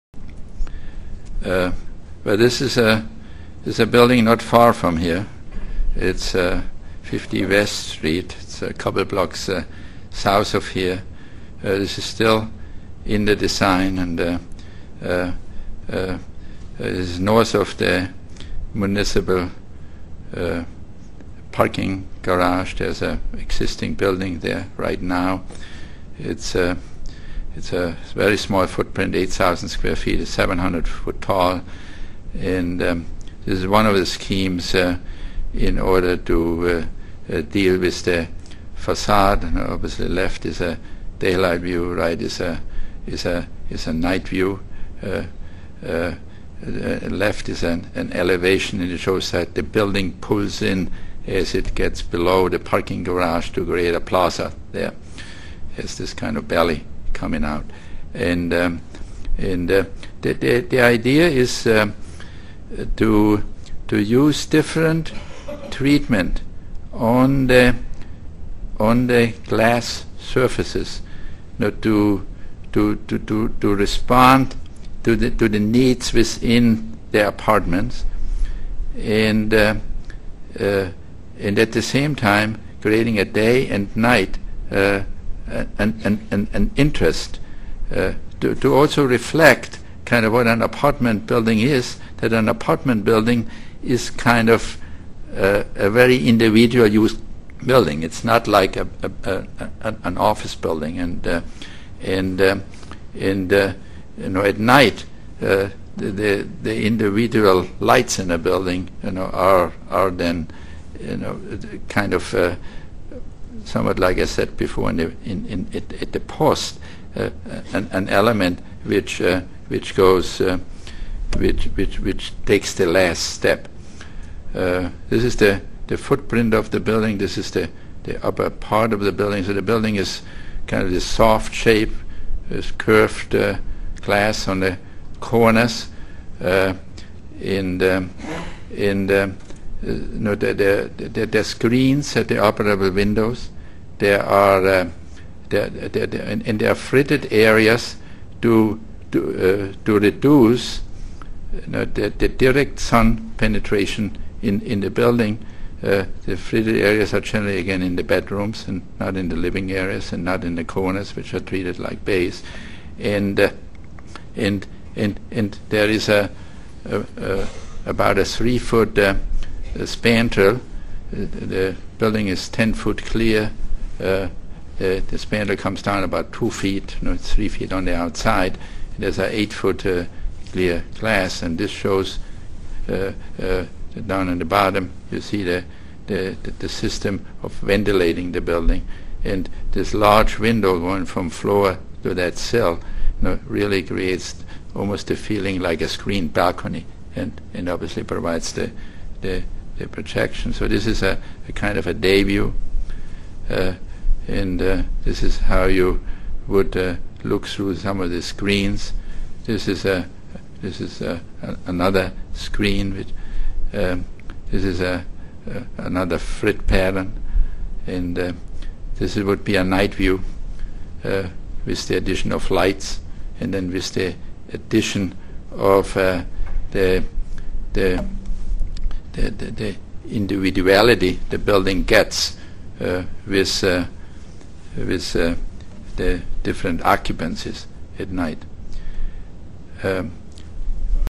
Helmut Jahn, Murphy/ Jahn: Part 8, 50 West, New York City
Helmut Jahn at The Skyscraper Museum
Mixed Greens: An International Survey of State-Of-The-Art Sustainable Skyscraper Design

HELMUT JAHN, President and CEO, Director of Design, Murphy/Jahn
Archi-Neering
Marrying engineering efficiencies and structural solutions with dynamic material design has made Helmut Jahn an innovator in energy-conscious high-rises, including the Deutsche Post Tower in Bonn, Germany, and an affordable housing project in Chicago.